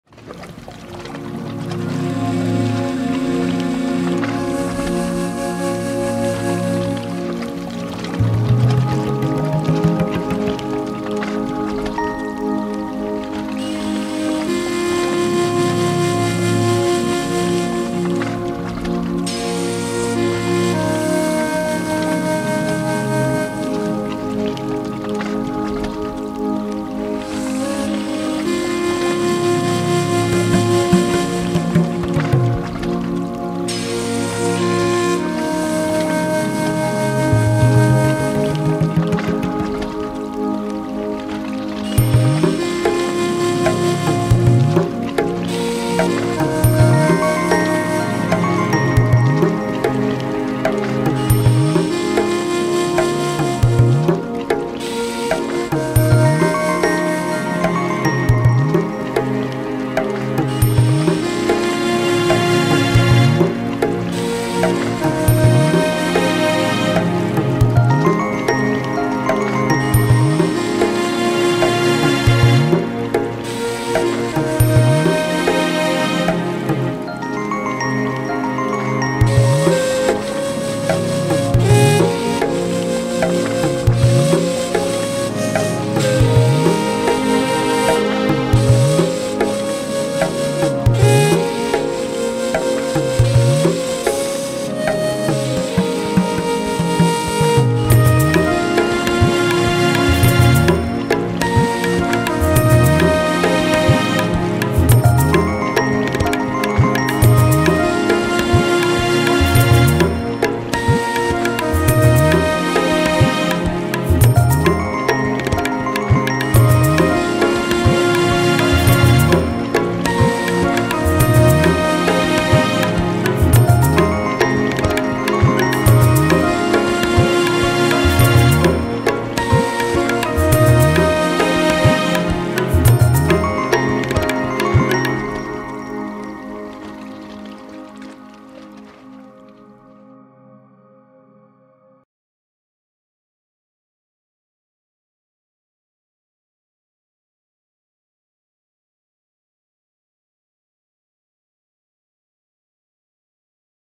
LITTORAL OUEST BEJAIA ALGERIE (Tighremt-Saket-Oued Dass-Beni Ksila)
Survol du littoral de la côte ouest de BEJAIA.
Boulimat..Tighremt..Saket..Oued dass..Beni Ksila...